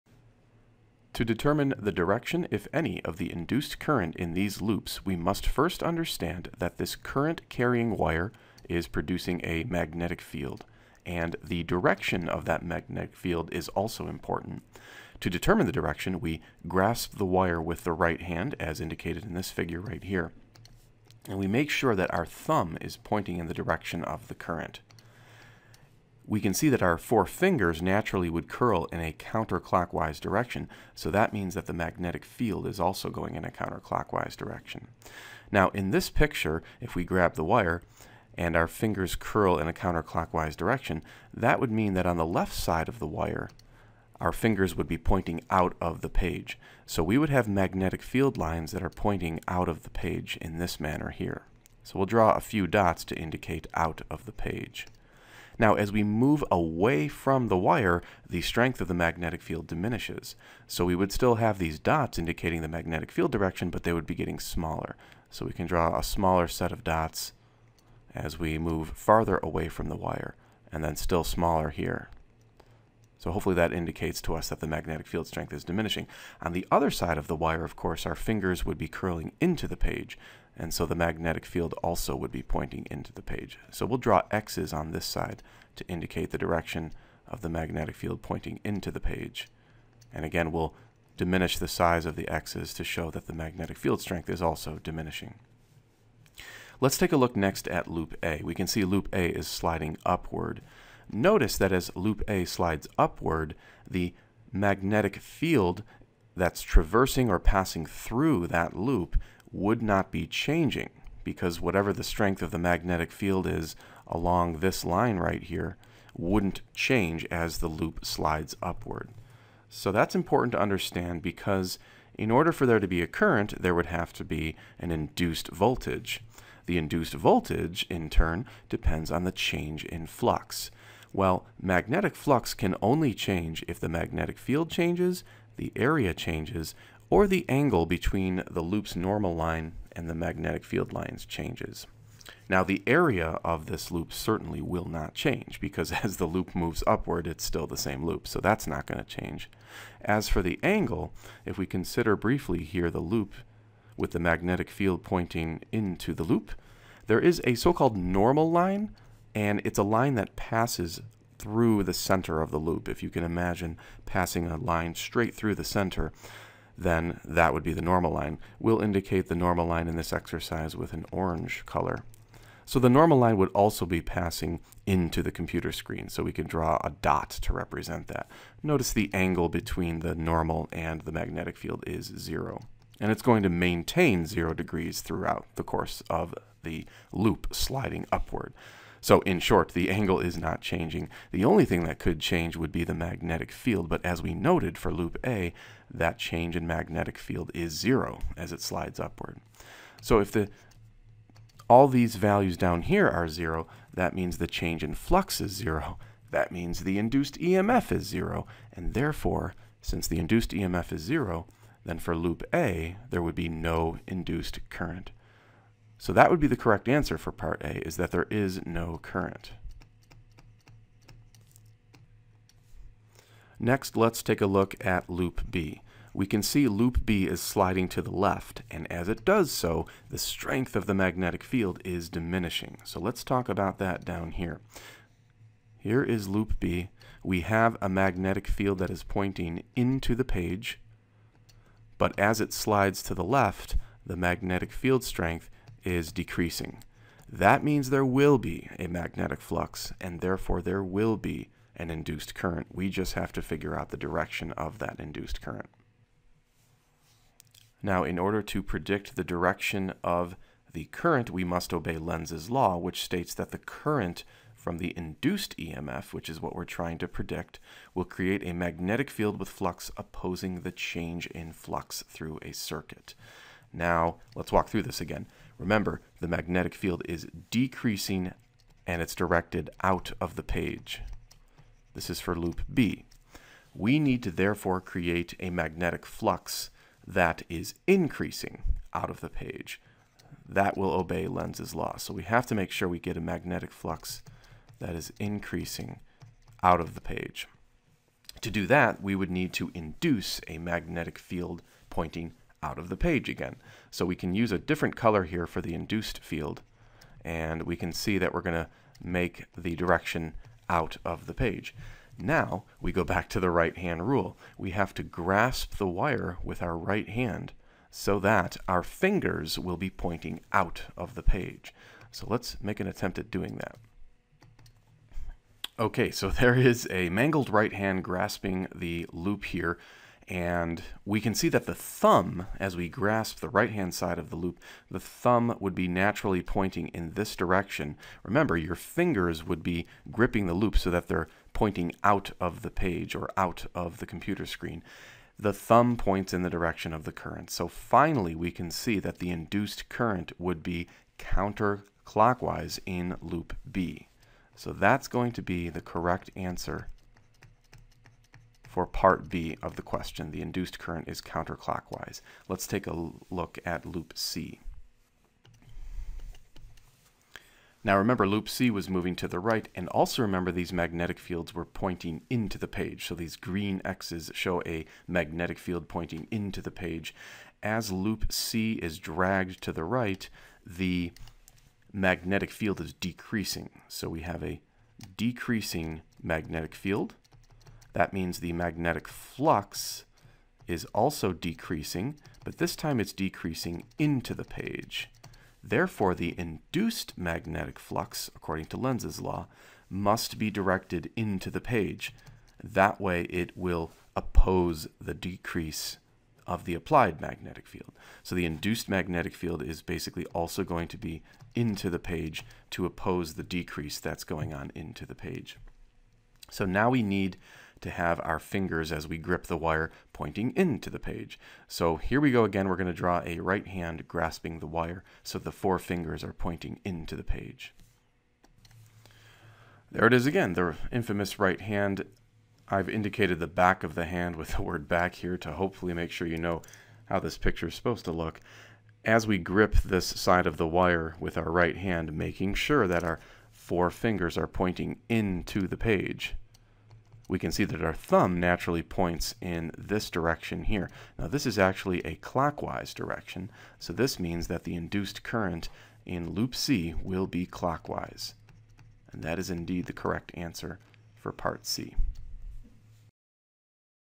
Three loops of wire move near a long
Three loops of wire move near a long straight wire
carrying a current as in the following figure. What is the direction of the induced current, if any, in (a) loop A, (b) loop B, and (c) loop C.